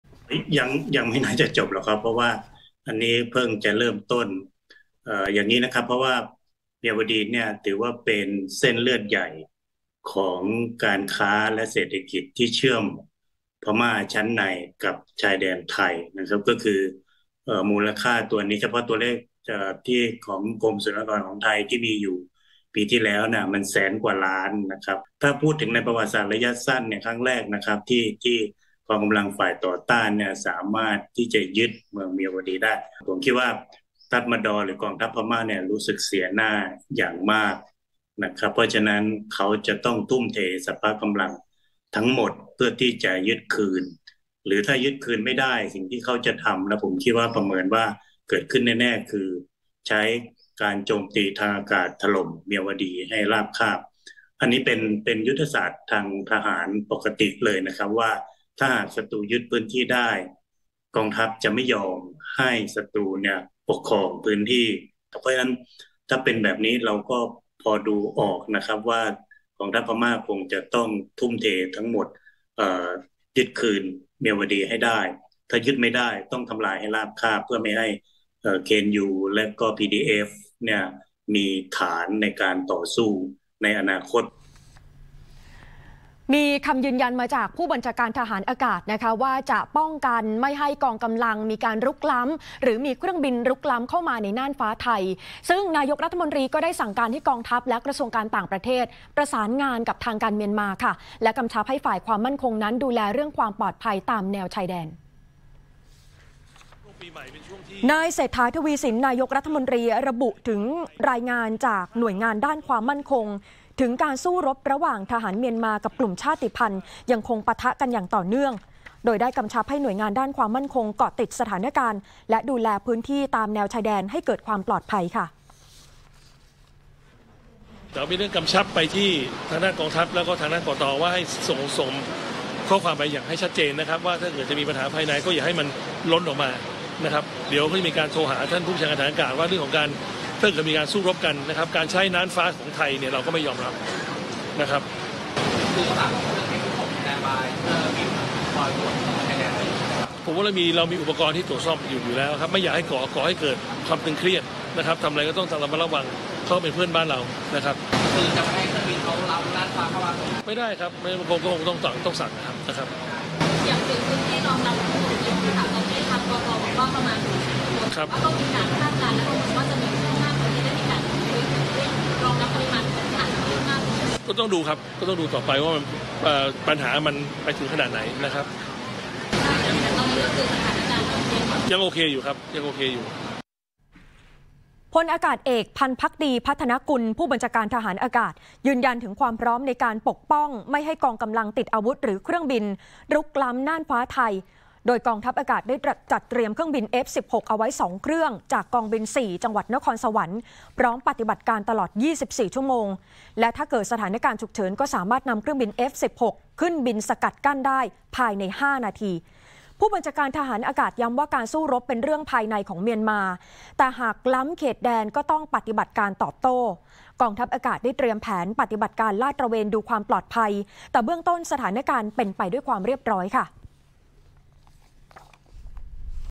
ประเมินการรบ หลังฝ่ายต่อต้านยึดเมียวดี สงครามจะเป็นอย่างไรต่อ? | Thai PBS News
สุภลักษณ์ กาญจนขุนดี นักข่าวอาวุโส และผู้เชี่ยวชาญด้านเอเชียตะวันออกเฉียงใต้ศึกษา ประเมินการรบว่า แม้กลุ่มต่อต้านจะยึดจังหวัดเมียวดี แต่ยังไม่สามารถควบคุมสถานการณ์ได้ กองทัพเมียนมาจะตอบโต้แน่นอน หากยึดกลับคืนไม่ได้ โฉมหน้าของการรบจะออกมาแบบไหน ติดตามจากคลิปนี้